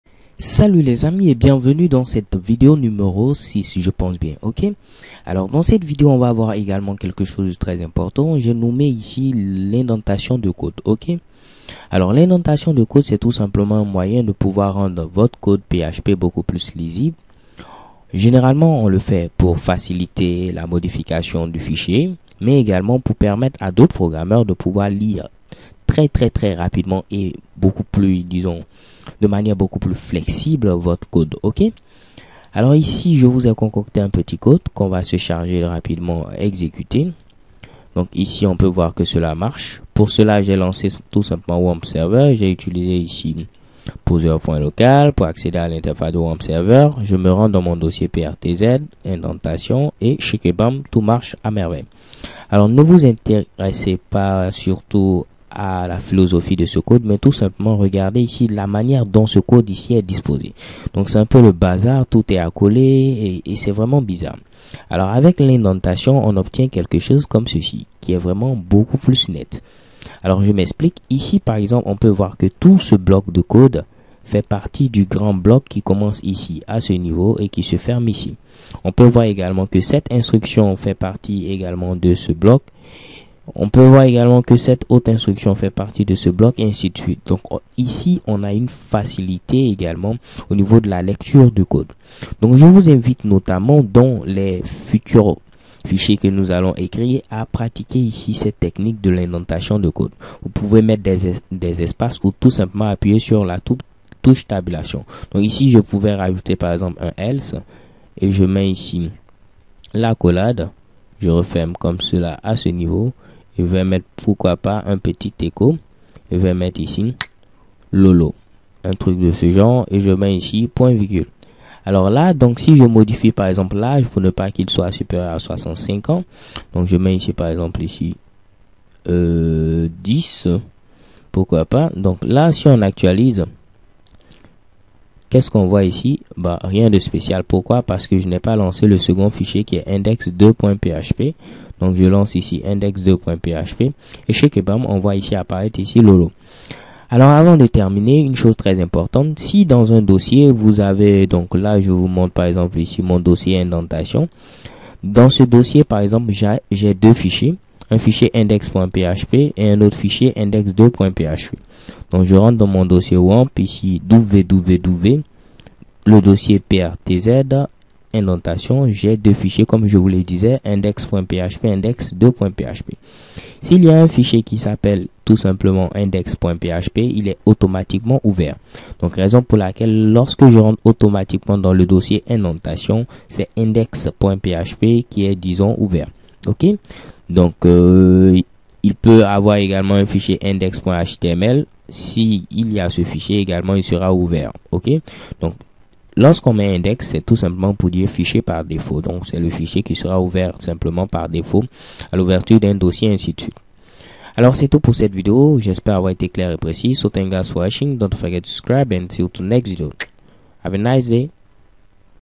PHP - 6 - Indentation de code
Comment rendre son code source plus lisible ? La réponse dans cette vidéo :)